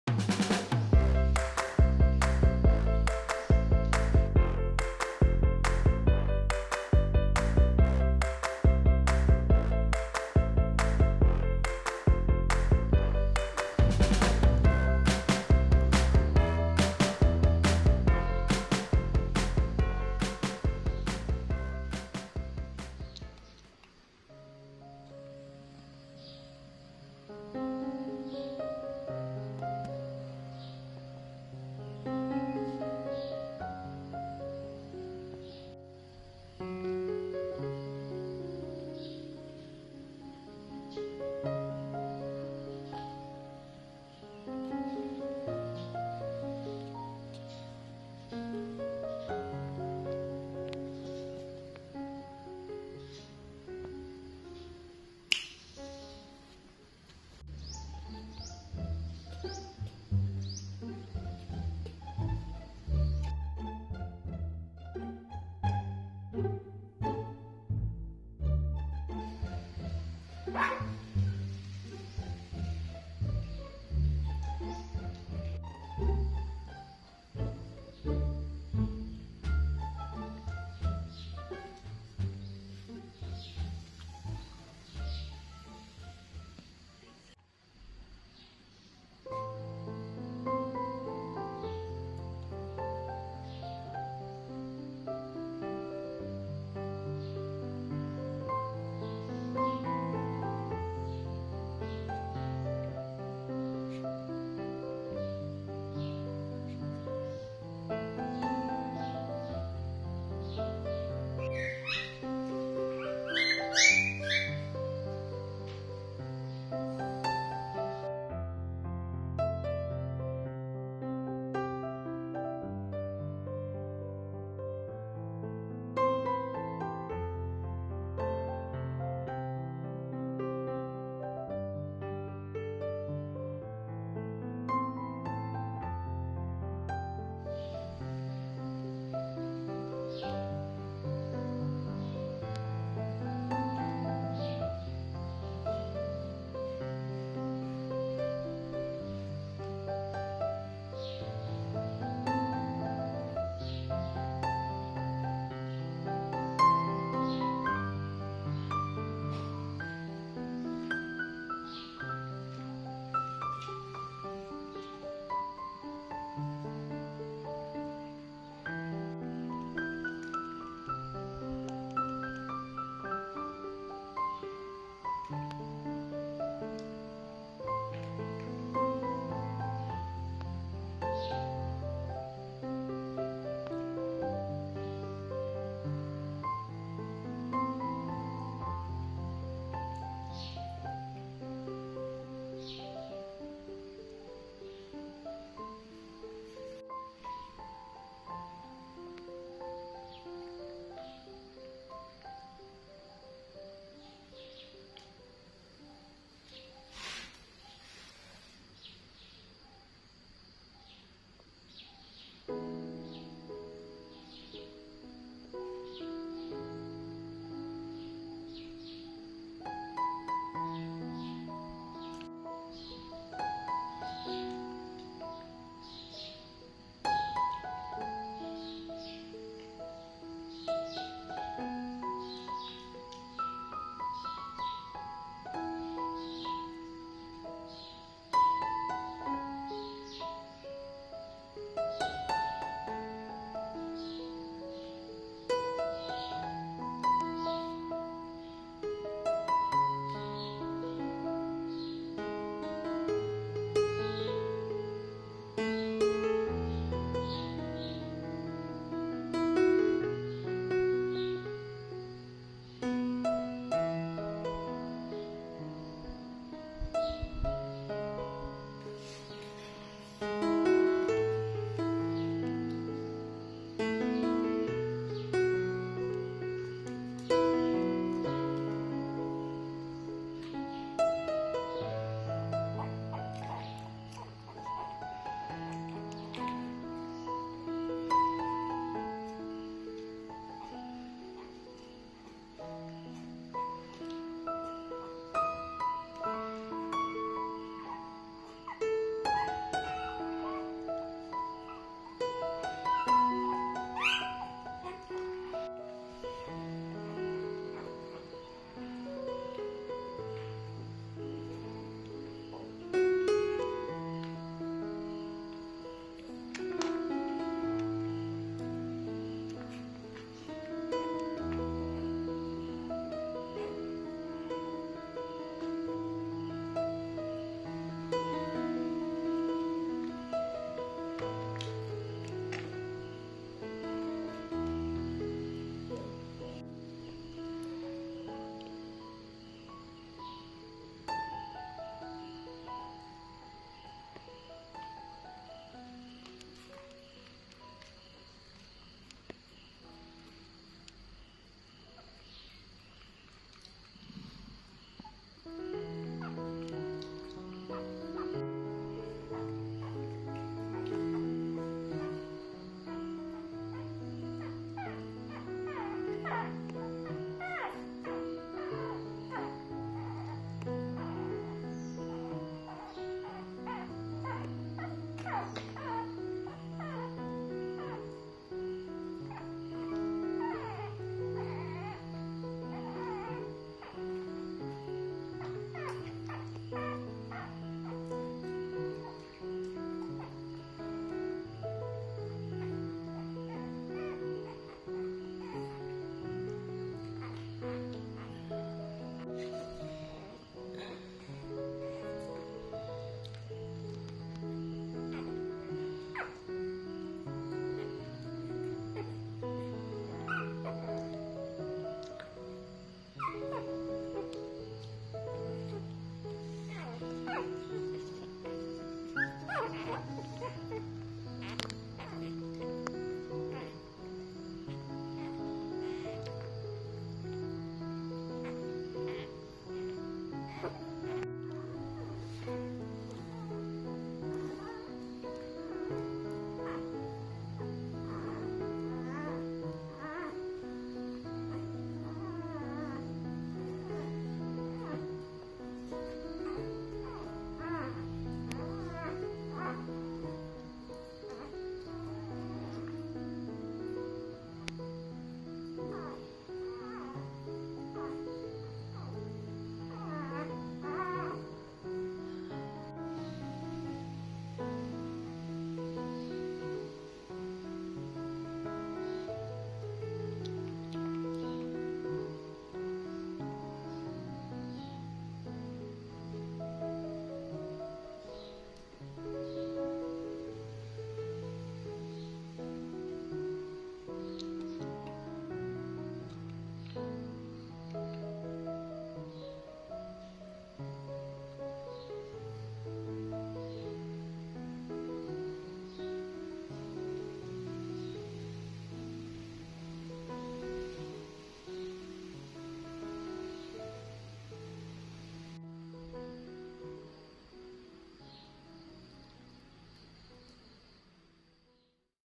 Super cute! Baby monkey Su returned to the puppies, surprising the mother dog
So cute! Baby monkey Su returned to the puppies, surprising the mother dog.
Welcome to KUKU Monkey channel!
Channel sharing about the daily life of KUKU monkey!